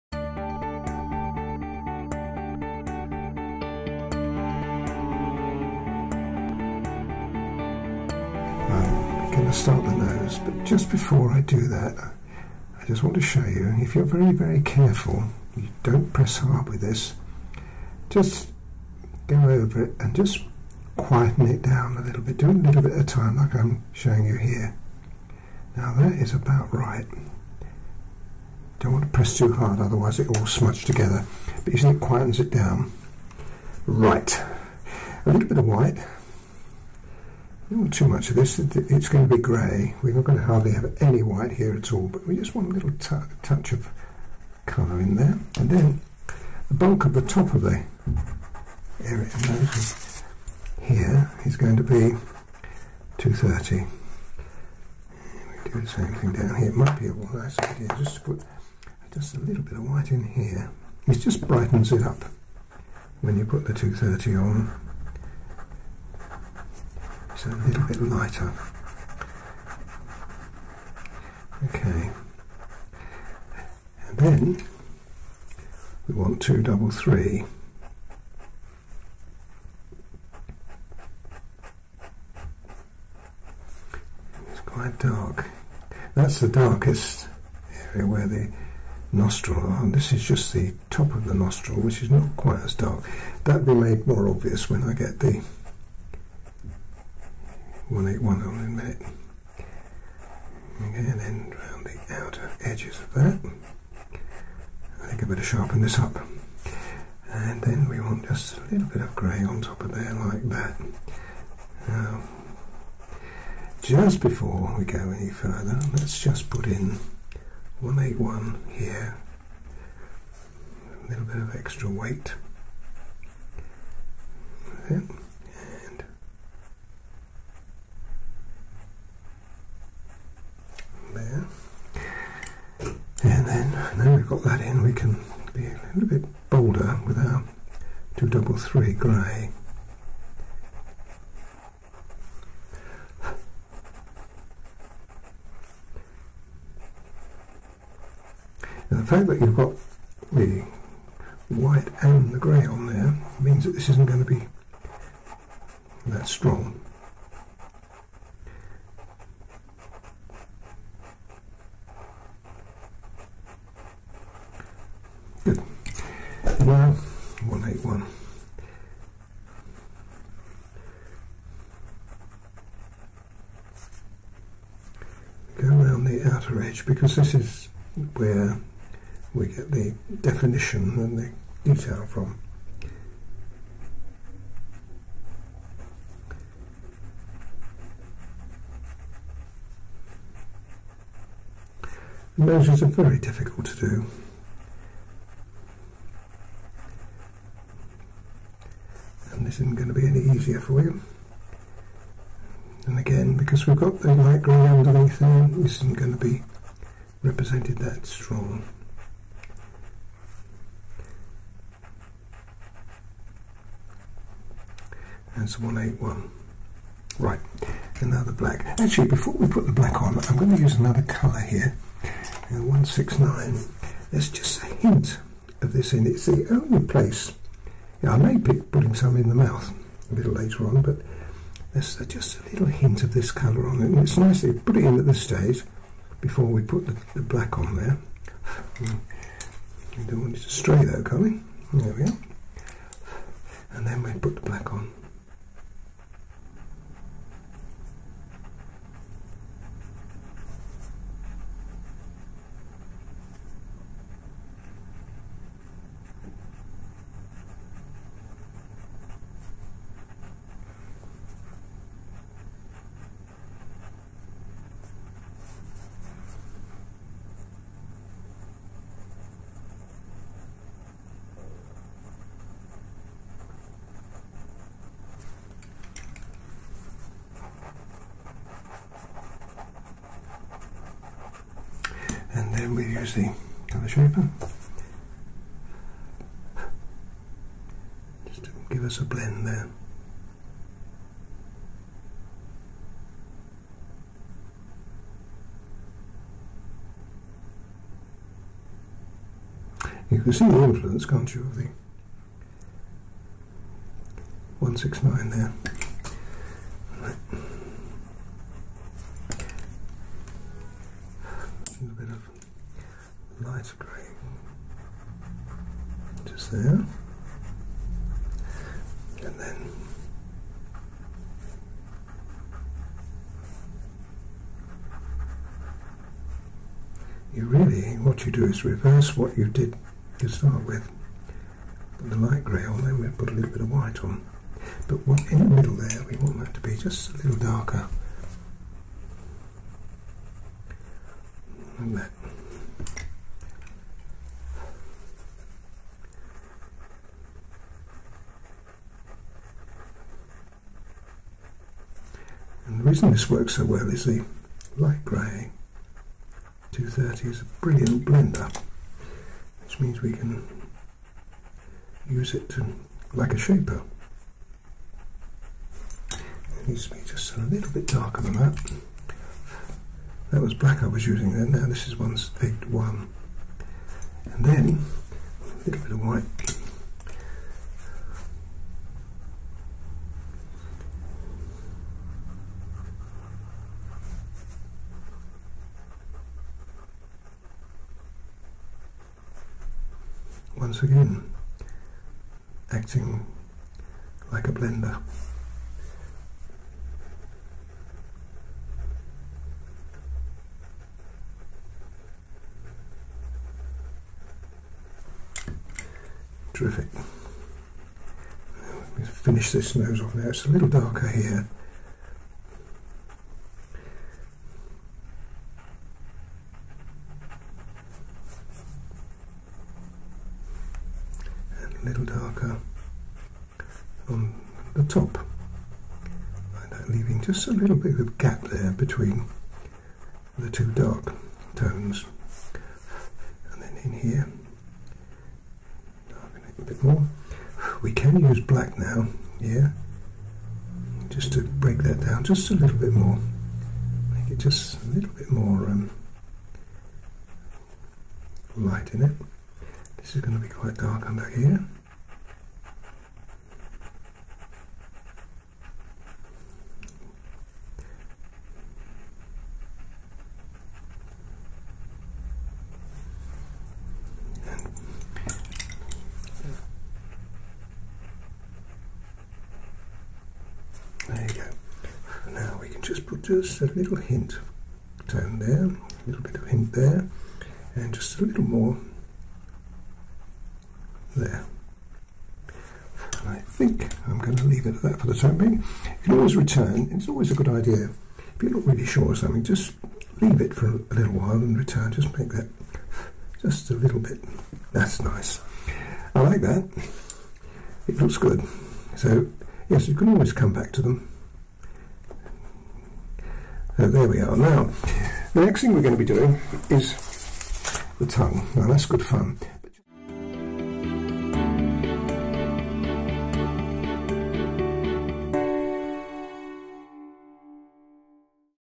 Afghan Hound Nose Tutorial | Pastel Pencils
In this quick clip watch Colin demonstrate how to draw the nose of this Afghan Hound.